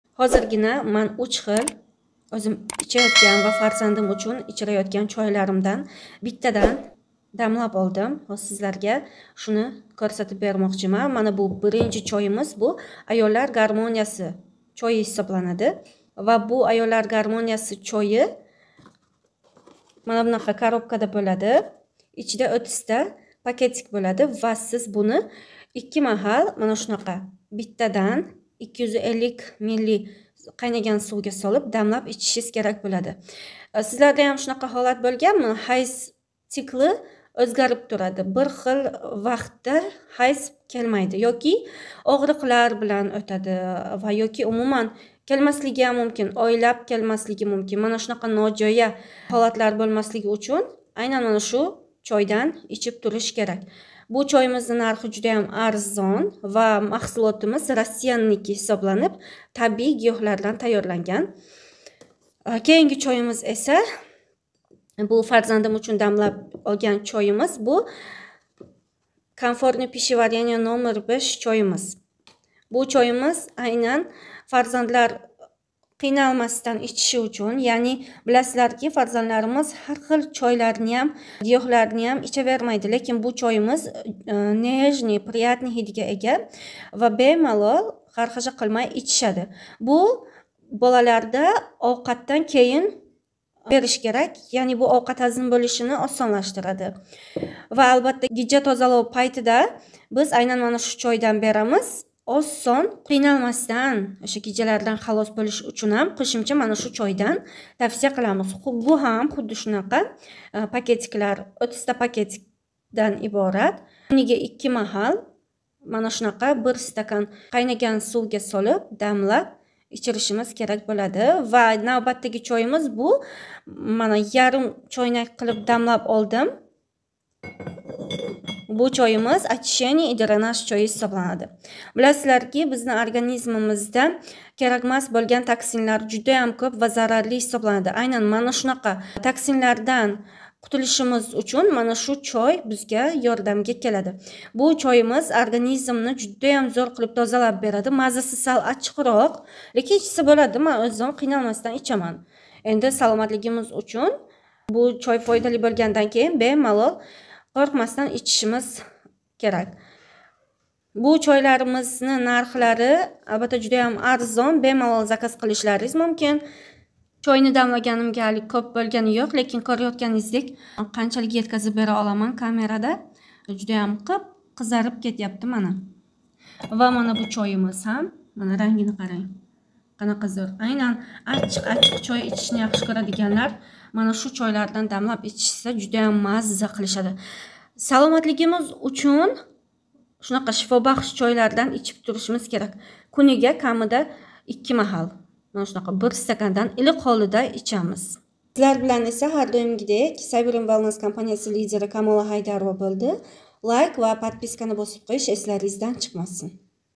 SIZ UCHUN MAXSUS SHIFOBAXSH CHOYLAR/ BU CHOYLARDAN ALBATTA OLIB ICHING
koʻk choy vitaminlarizni yuvib chiqaradi. bu choylar esa nabarot sogligʻiz uchun gʻamxoʻrlik qiladi kanalimga obuna bolib like bosishni unutmang